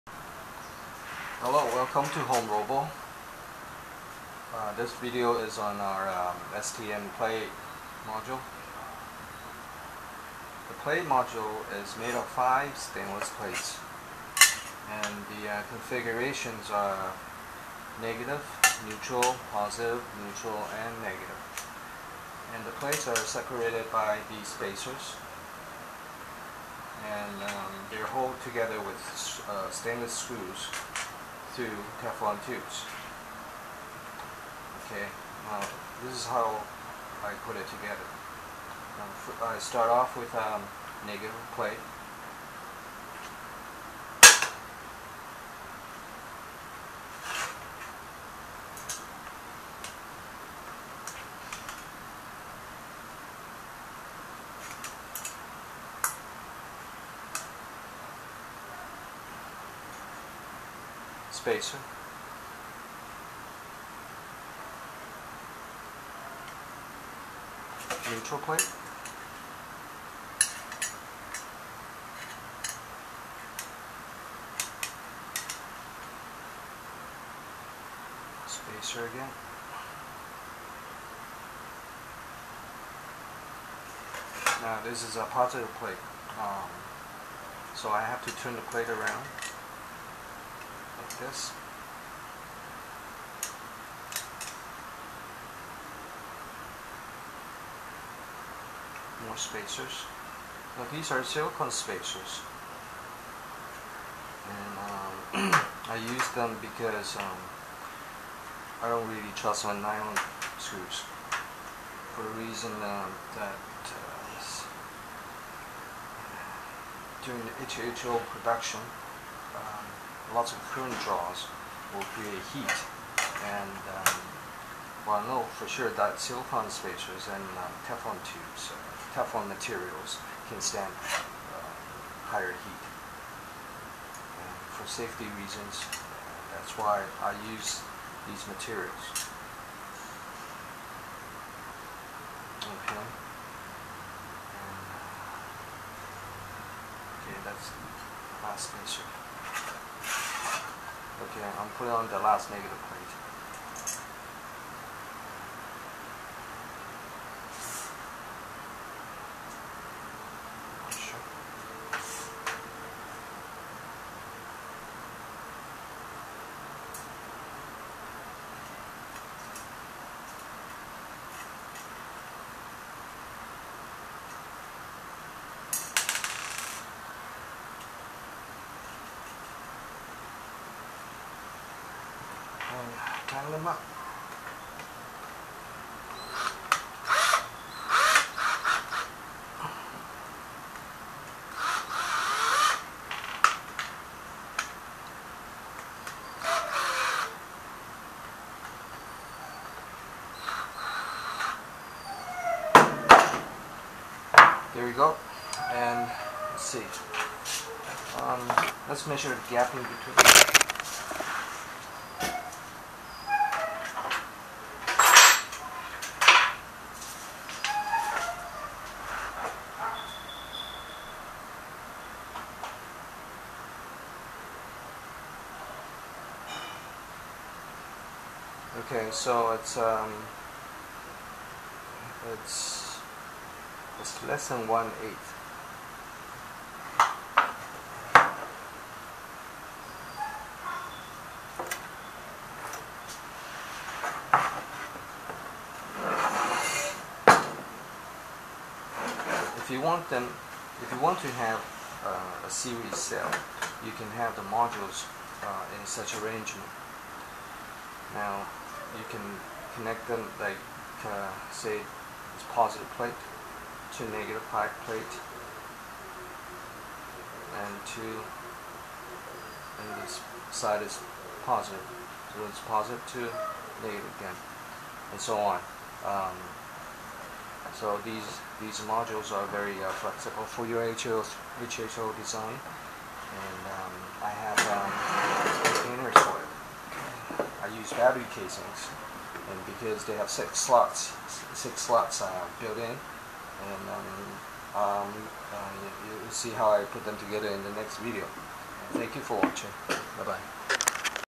HomeRobo Stainless Plate Module for HHO Projects
Plate sizes are 6in X 4in for positve and negative plates and 5in X 4in for neutual plates. they are 1.5mm thick. total weight approx 670 grams.